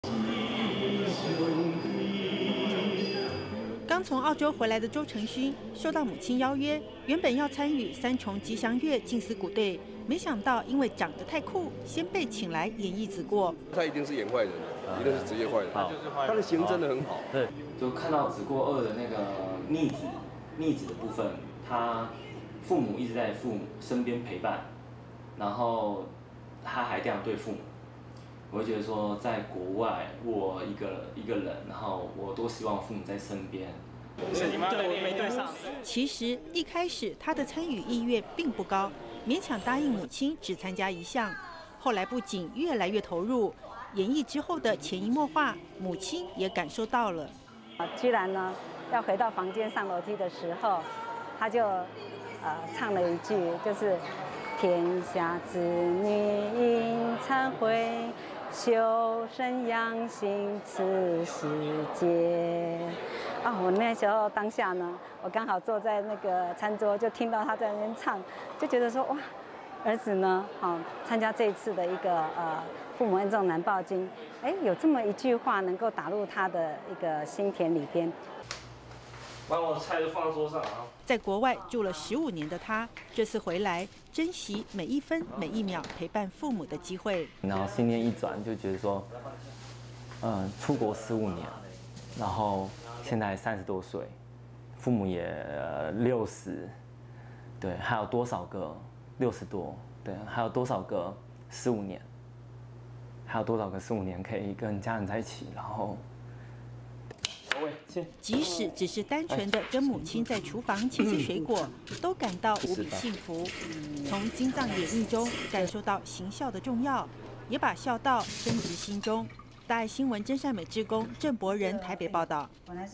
吉祥演繹 看見心轉變
剛從澳洲回來的周呈勳，參加三重吉祥月活動，演繹父母恩重難報經的子過片段，沒想到卻因此潛移默化，自己的改變，母親也深深感受到了。